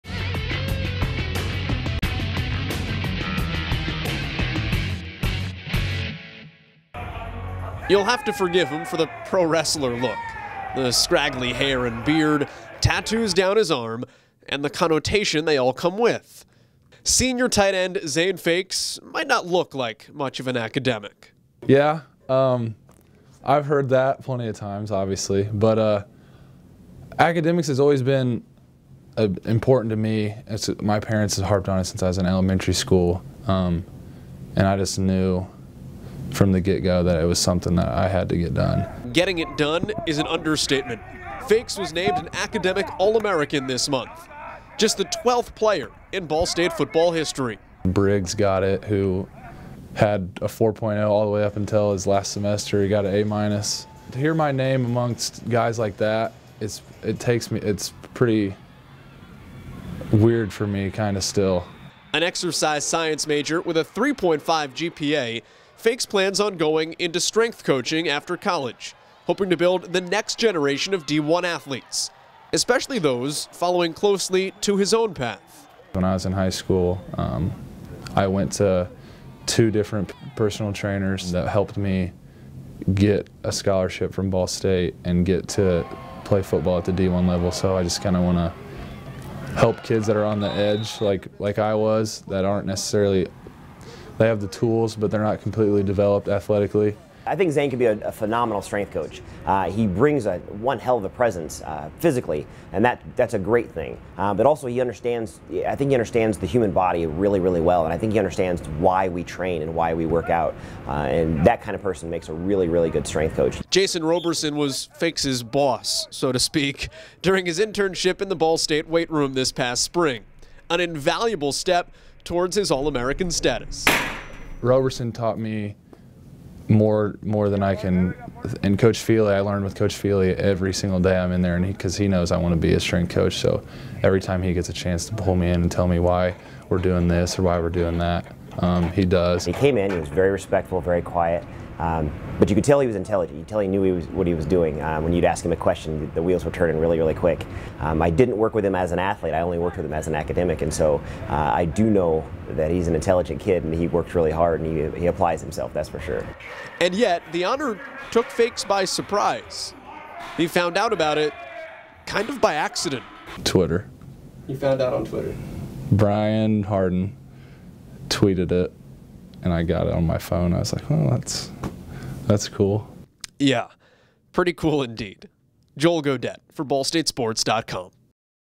Ball State's Zane Fakes Named Academic All-American
Ball State senior tight end Zane Fakes was named an Academic All-America 1st Team performer in 2013.  Fakes is the 12th Ball State player to earn the honor in program history.